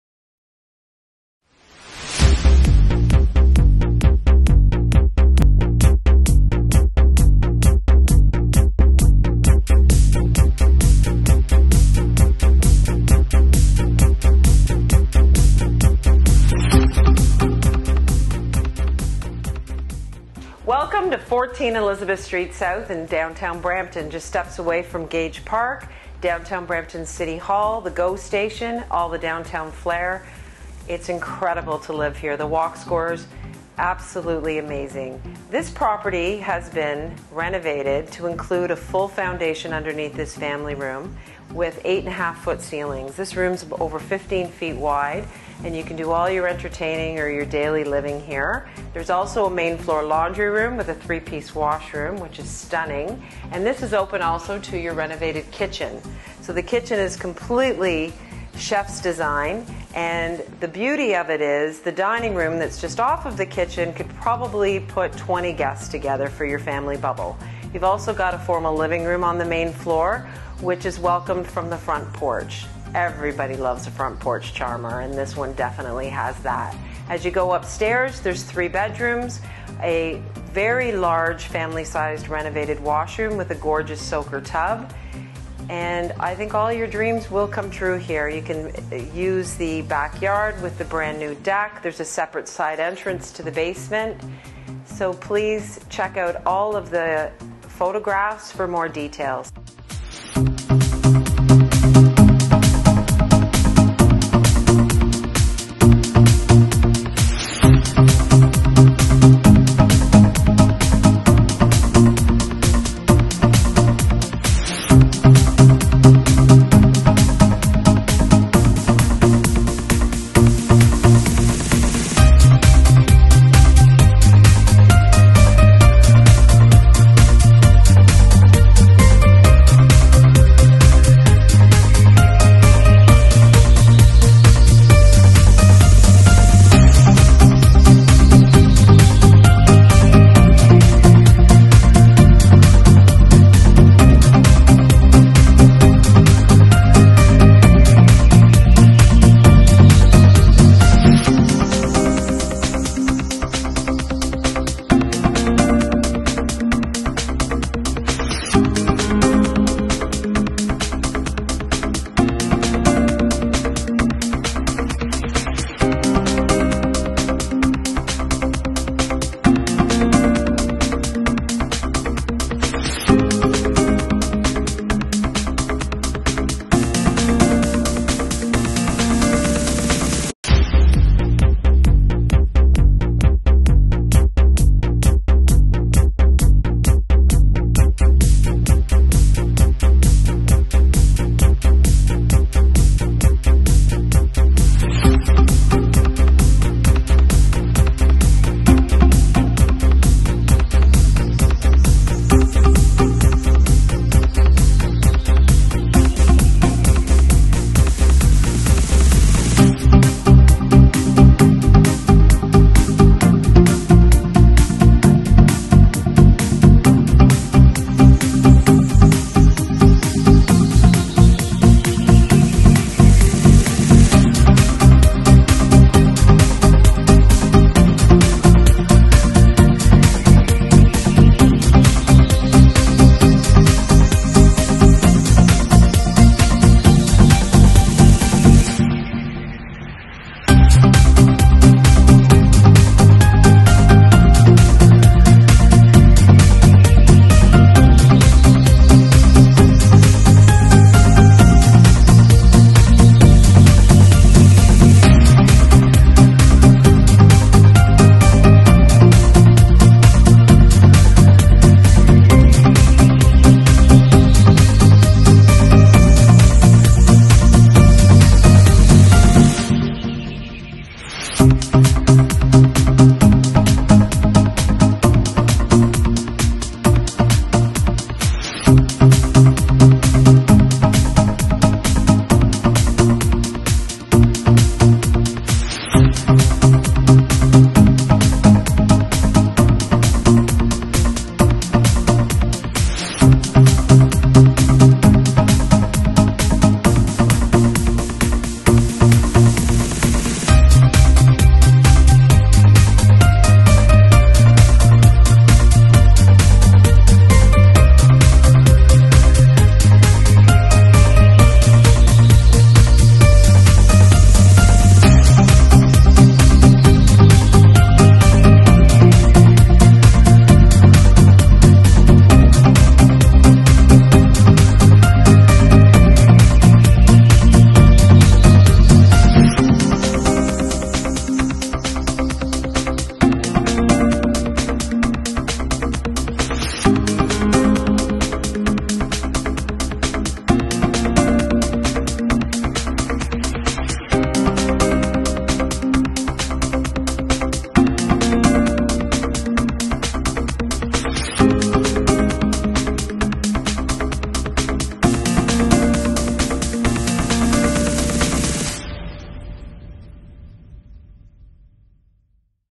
14 Elizabeth Street South, Brampton
Top 5 Reasons To Set Your Roots & Invest In Your Future At 14 Elizabeth St S. 1.Walk Score To Downtown Flare-Gage Park,City Hall,Go Station,Rose Theater,Etobicoke Creek Trails To Bike & Hike 2.Awesome Renovated Living Space W/Exquisite Detailed Carpentry & Craftsmanship 3.Front & Back Porch Added Living Space 4.Turn Key Condition With Modern Amenities;Main Flr Laundry & Leisure Luxury Washrooms 5.Sep Ent To Full 8.5' High Lower Level Under Great Rm